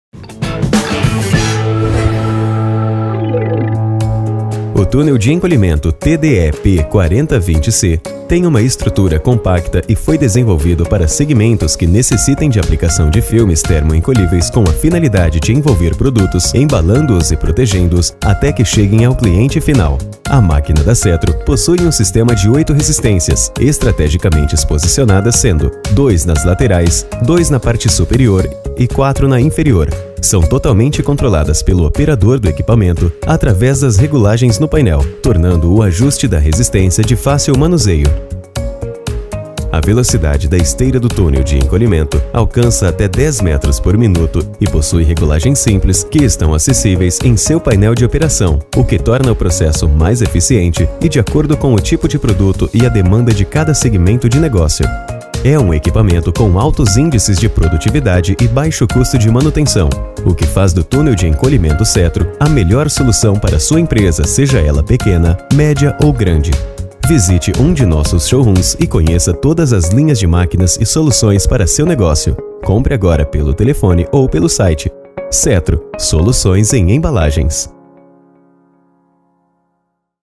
Túnel de Encolhimento TDE-P 4020C
O túnel de encolhimento TDE-P4020C, tem uma estrutura compacta e foi desenvolvido para segmentos que necessitem da aplicação de filmes termo encolhíveis com a finalidade de envolver produtos, embalando-os e protegendo-os até que cheguem ao cliente final.

A máquina da Cetro, possui um sistema de 8 resistências, estrategicamente posicionadas, sendo: 2 nas laterais, 2 na parte superior e 4 na inferior, são totalmente controladas pelo operador do equipamento através de regulagens no painel, tornando o ajuste da resistência de fácil manuseio.